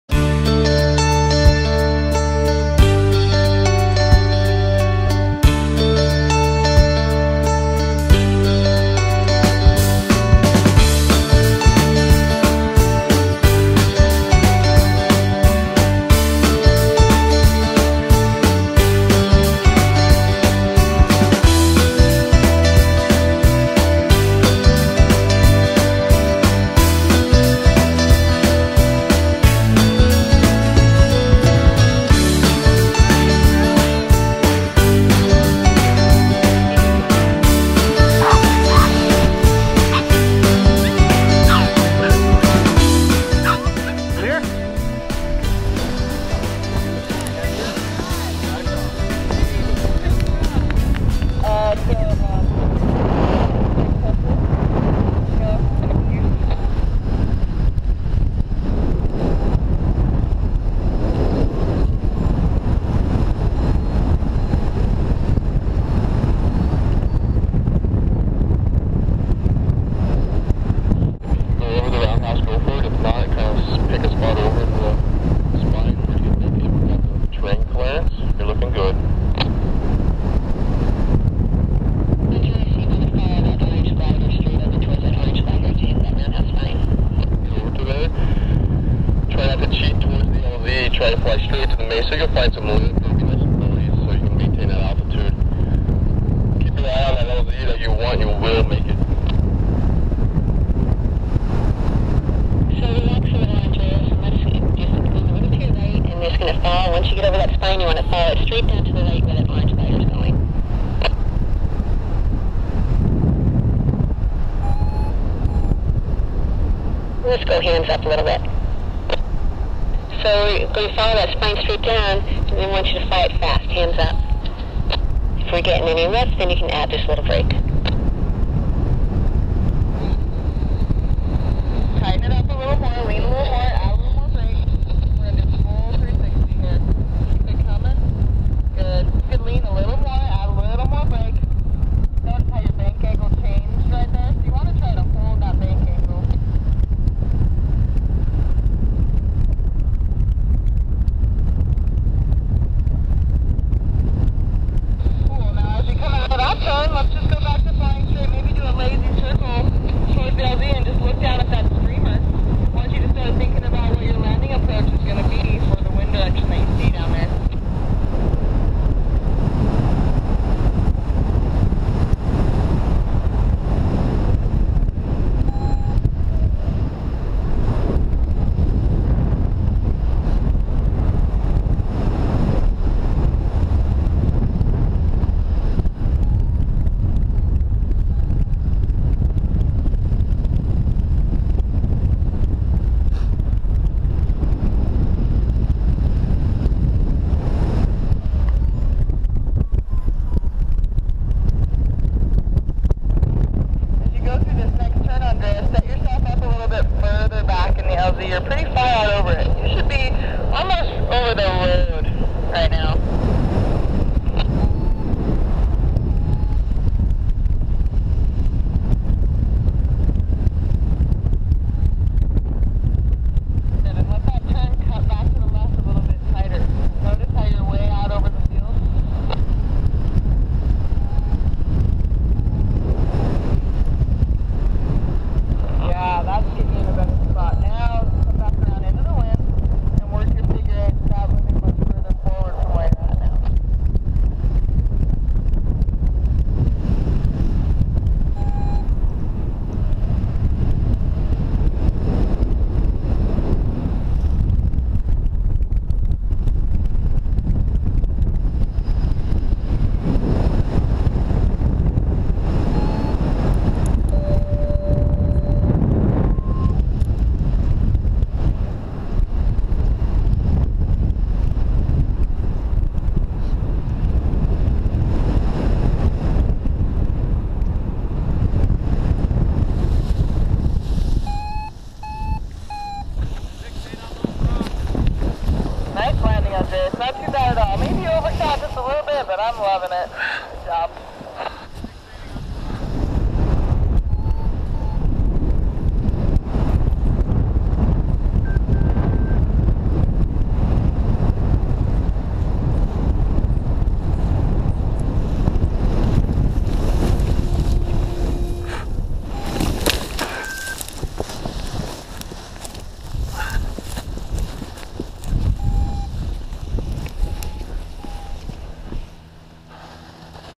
My First Mountain Flights
My (Andreas Leitner) First Mountain Flights. I was pretty nervous and made lot's of mistakes that I thought I left behind on the training hill, but all went fine -- and it was amazing.